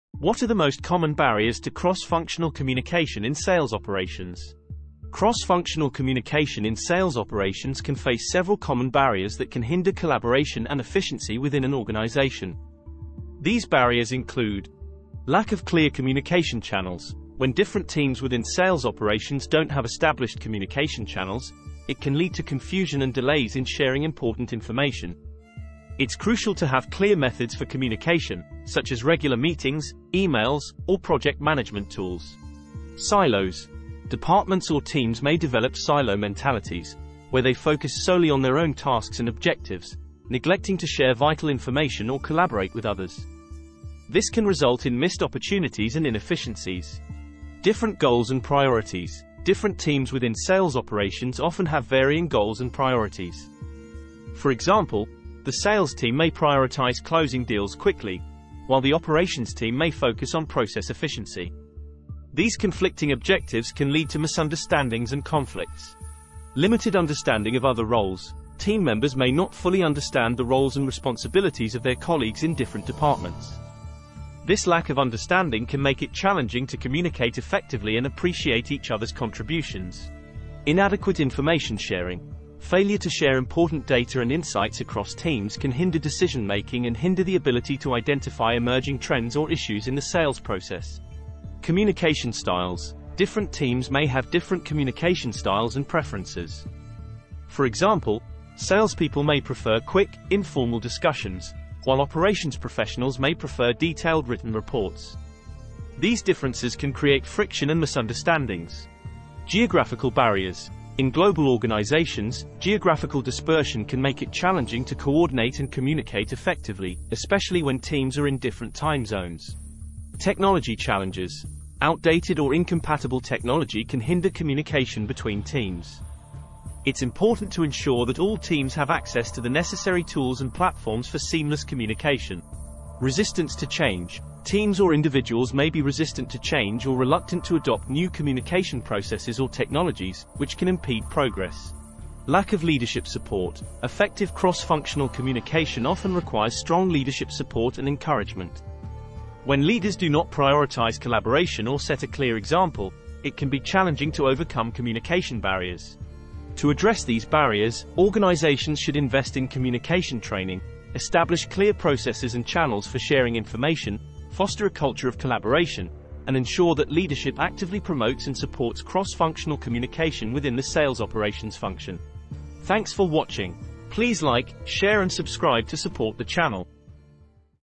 What are the most common barriers to cross-functional communication in Sales Operations?
Cross-functional communication in Sales Operations can face several common barriers that can hinder collaboration and efficiency within an organization. These barriers include:

Lack of Clear Communication Channels: When different teams within Sales Operations don't have established communication channels, it can lead to confusion and delays in sharing important information. It's crucial to have clear methods for communication, such as regular meetings, emails, or project management tools.

Silos: Departments or teams may develop silo mentalities, where they focus solely on their own tasks and objectives, neglecting to share vital information or collaborate with others. This can result in missed opportunities and inefficiencies.

Different Goals and Priorities: Different teams within Sales Operations often have varying goals and priorities. For example, the sales team may prioritize closing deals quickly, while the operations team may focus on process efficiency. These conflicting objectives can lead to misunderstandings and conflicts.

Limited Understanding of Other Roles: Team members may not fully understand the roles and responsibilities of their colleagues in different departments. This lack of understanding can make it challenging to communicate effectively and appreciate each other's contributions.

Inadequate Information Sharing: Failure to share important data and insights across teams can hinder decision-making and hinder the ability to identify emerging trends or issues in the sales process.

Communication Styles: Different teams may have different communication styles and preferences. For example, salespeople may prefer quick, informal discussions, while operations professionals may prefer detailed written reports. These differences can create friction and misunderstandings.

Geographical Barriers: In global organizations, geographical dispersion can make it challenging to coordinate and communicate effectively, especially when teams are in different time zones.

Technology Challenges: Outdated or incompatible technology can hinder communication between teams. It's important to ensure that all teams have access to the necessary tools and platforms for seamless communication.

Resistance to Change: Teams or individuals may be resistant to change or reluctant to adopt new communication processes or technologies, which can impede progress.

Lack of Leadership Support: Effective cross-functional communication often requires strong leadership support and encouragement. When leaders do not prioritize collaboration or set a clear example, it can be challenging to overcome communication barriers.

To address these barriers, organizations should invest in communication training, establish clear processes and channels for sharing information, foster a culture of collaboration, and ensure that leadership actively promotes and supports cross-functional communication within the Sales Operations function.